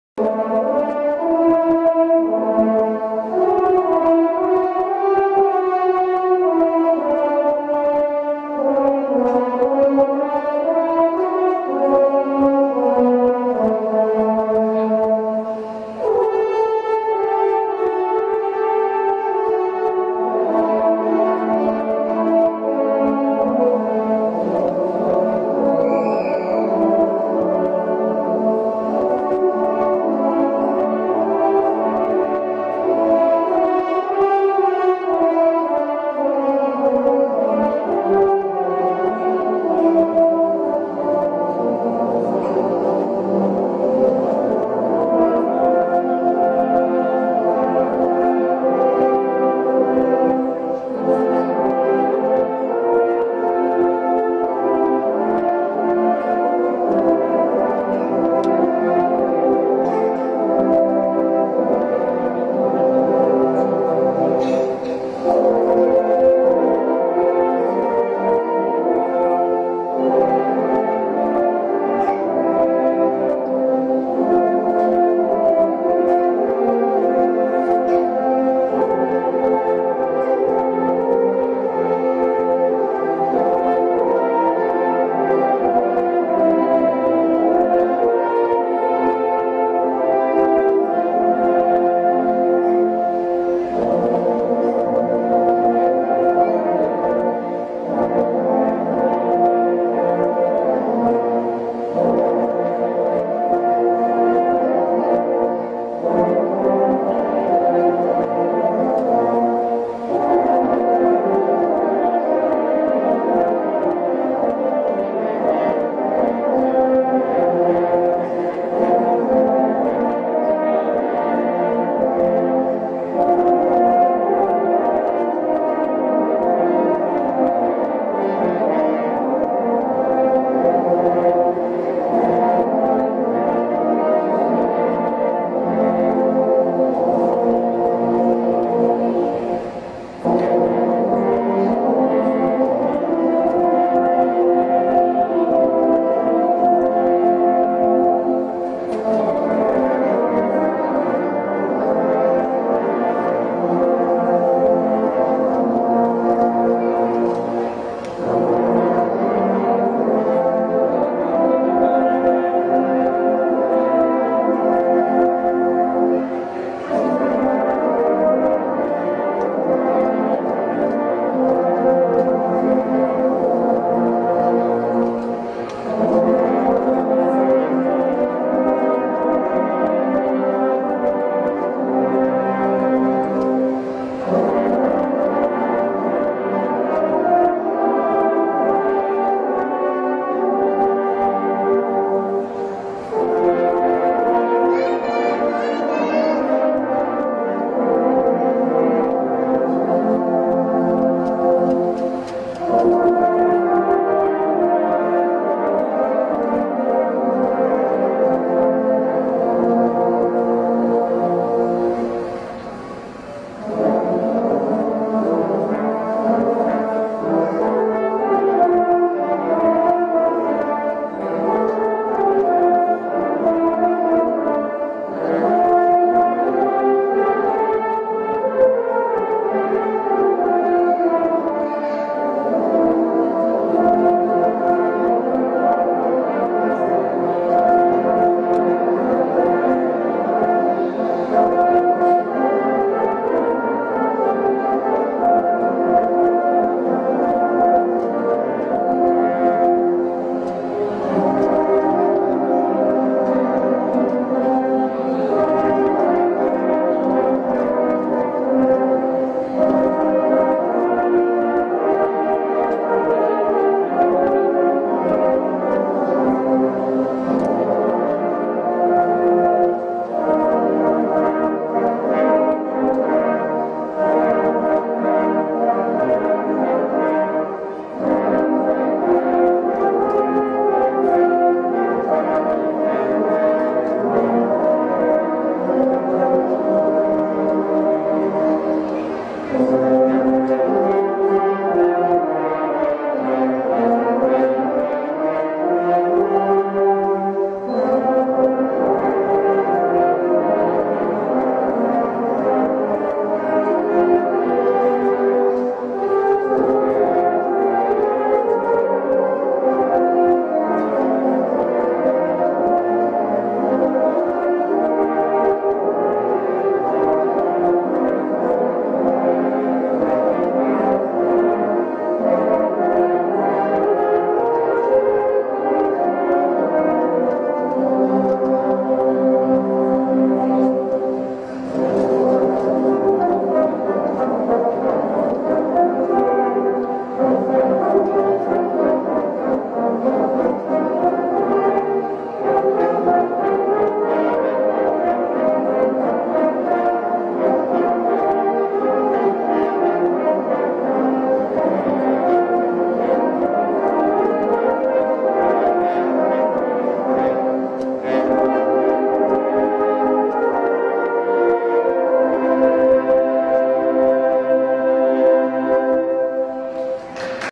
James Thaldorf Conducts Horns A Plenty 2003
James Thaldorf, director of bands at Appleton North High School, conducts a few tunes at Horns A Plenty Christmas 2003, Appleton. Guest soloists were Michael Stanley and Jeff Nelsen (can be seen in the very back of the ensemble).

Location: Appleton City Center, Appleton, WI